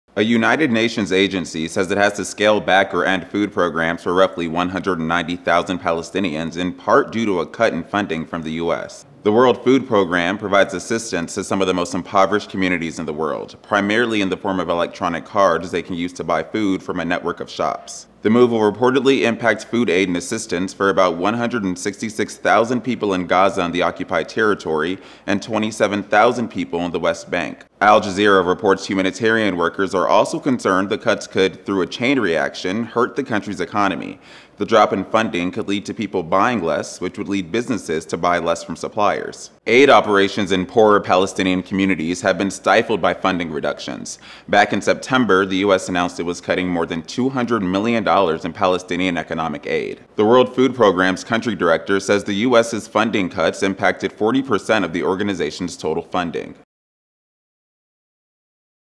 UN food program reducing aid to Palestinians
The U.N. World Food Program says it's reducing aid to about 190,000 Palestinians due to a drop in funding.